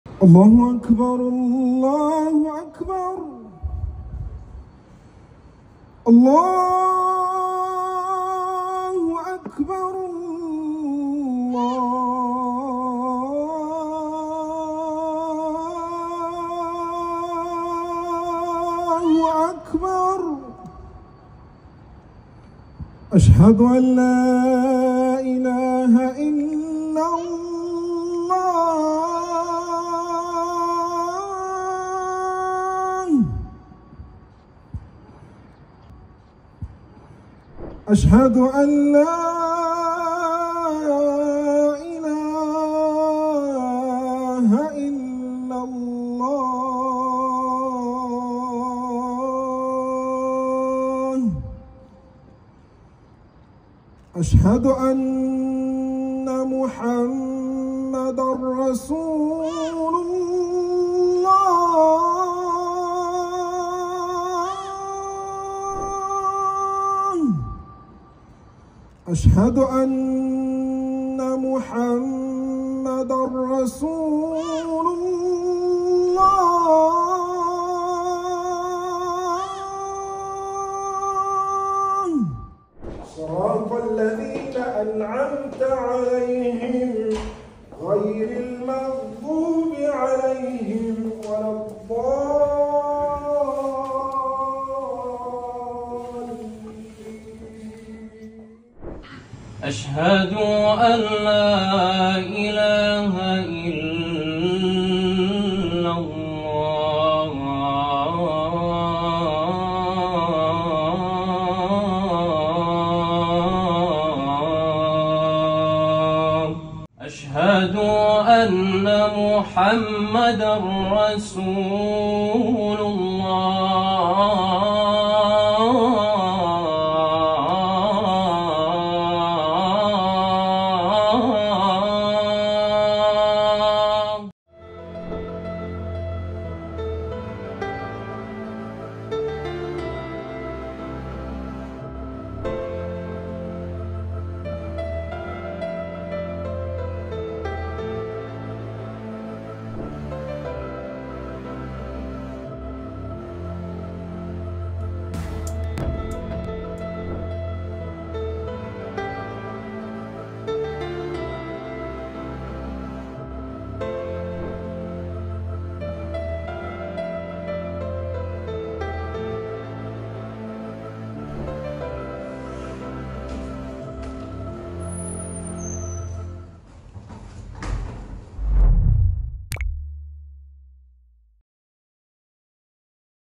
أبرزها مانشستر سيتي.. إفطار شهر رمضان ورفع أذان المغرب في ملاعب إنجلترا 🌙⚽
إفطار شهر رمضان 2024 في الملاعب الإنجليزية ضمن مبادرة الخيمة الرمضانية.